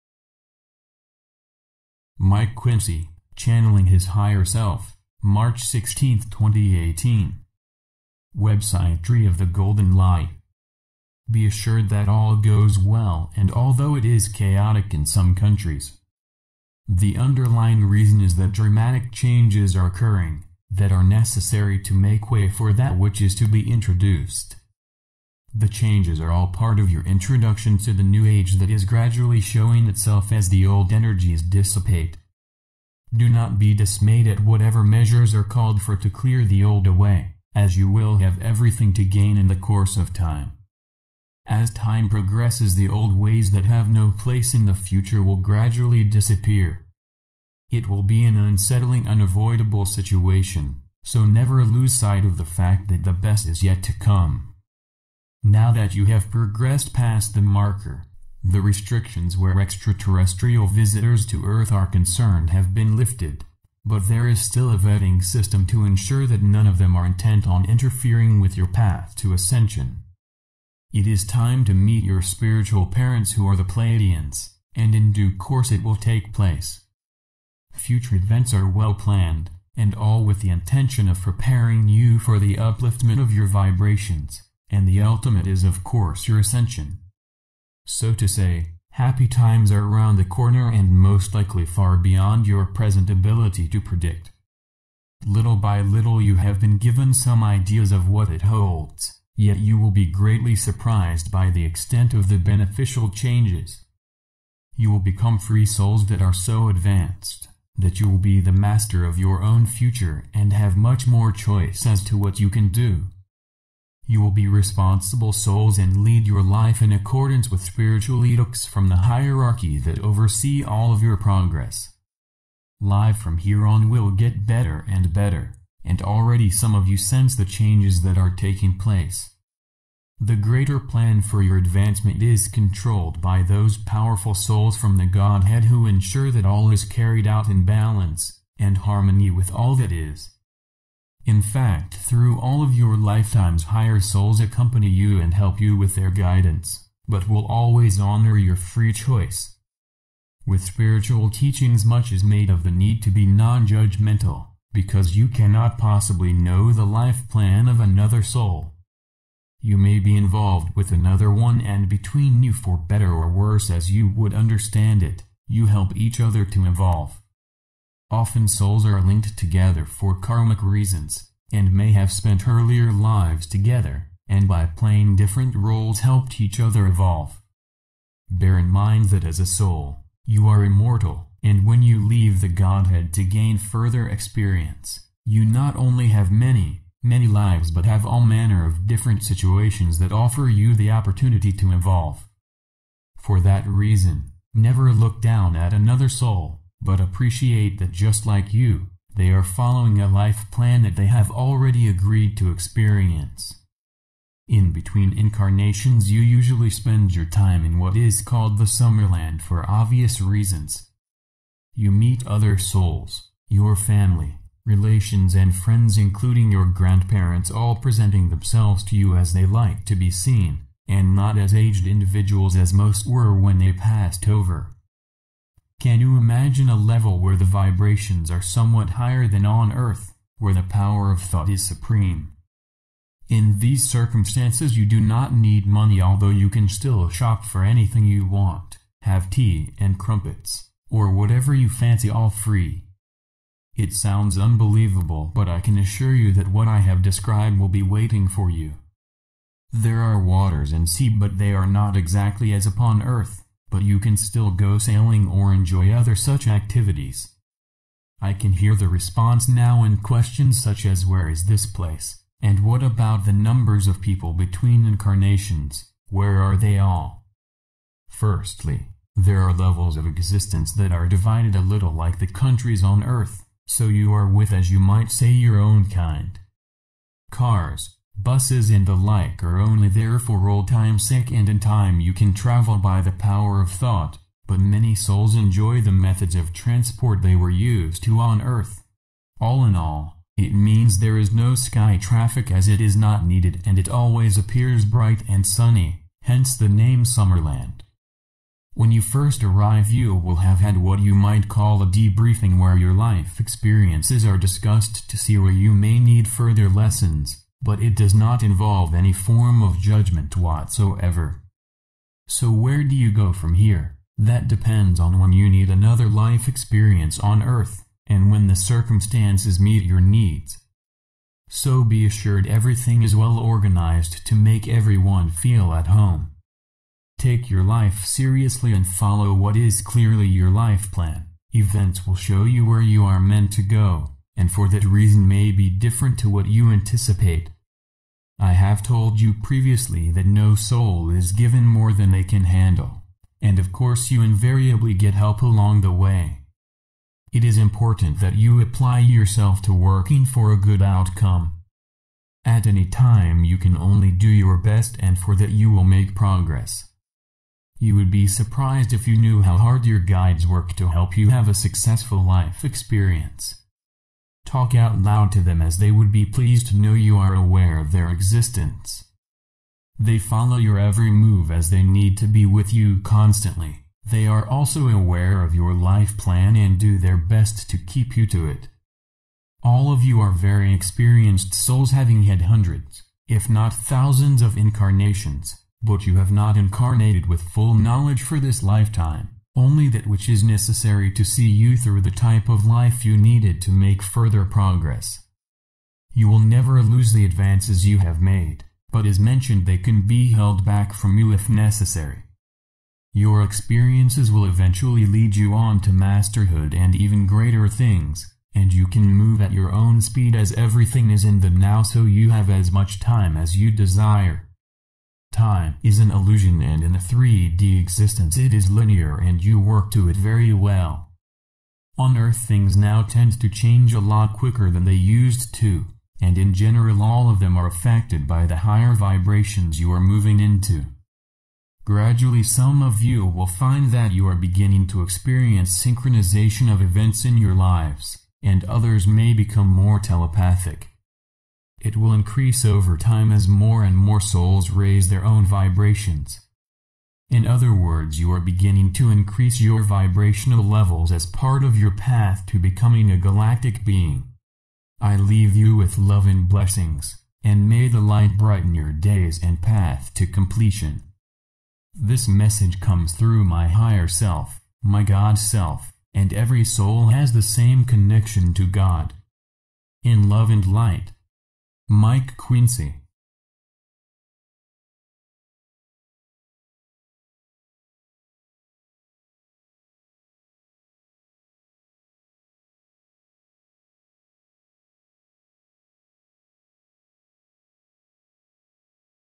Mike Quinsey ~ Channeling his Higher Self ~ 16 March 2018
Be assured that all goes well and although it is chaotic in some countries. The underlying reason is that dramatic changes are occurring, that are necessary to make way for that which is to be introduced. The changes are all part of your introduction to the New Age that is gradually showing itself as the old energies dissipate. Do not be dismayed at whatever measures are called for to clear the old away, as you will have everything to gain in the course of time. As time progresses the old ways that have no place in the future will gradually disappear. It will be an unsettling unavoidable situation, so never lose sight of the fact that the best is yet to come.

Now that you have progressed past the marker, the restrictions where extraterrestrial visitors to Earth are concerned have been lifted, but there is still a vetting system to ensure that none of them are intent on interfering with your path to Ascension. It is time to meet your “spiritual parents” who are the Pleiadians, and in due course it will take place. Future events are well planned, and all with the intention of preparing you for the upliftment of your vibrations, and the ultimate is of course your Ascension. So to say, happy times are around the corner and most likely far beyond your present ability to predict. Little by little you have been given some ideas of what it holds, yet you will be greatly surprised by the extent of the beneficial changes.